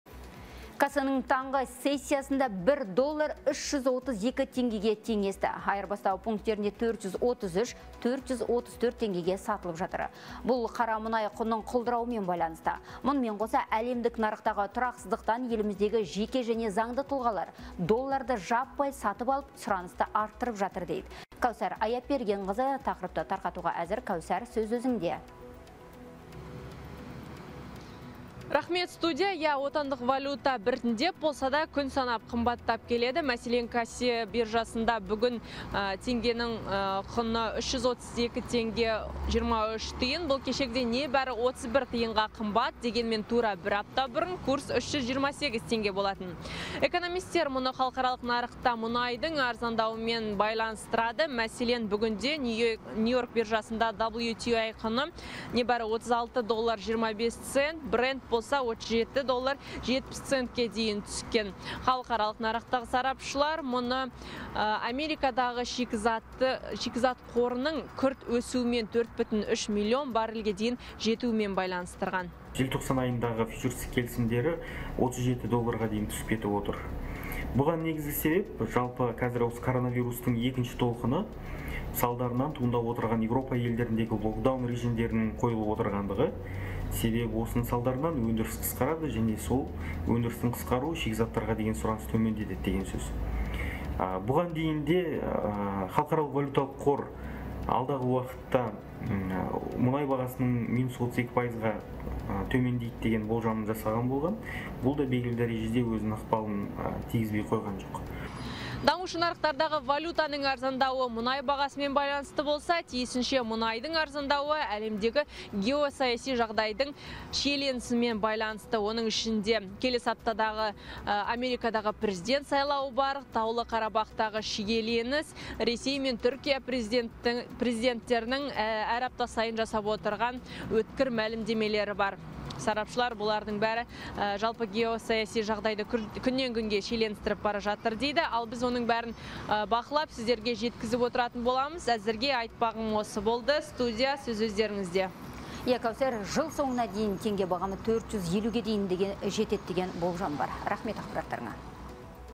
САЯСИ ТҰРАҚСЫЗДЫҚ САЛДАРЫНАН ДОЛЛАРҒА СҰРАНЫС АРТТЫ
Касенің таңғы сессиясында 1 доллар 332 теңгеге теңесті. Айырбастау пунктерінде 433-434 теңгеге сатылып жатыр. Бұл қара мұнай құнының құлдырауымен байланысты. Мұнымен қоса әлемдік нарықтағы тұрақсыздықтан еліміздегі жеке және заңды тұлғалар долларды жаппай сатып алып, сұранысты арттырып жатыр дейді. Кәусар Аяпбергенқызы тақырыпты толықтырады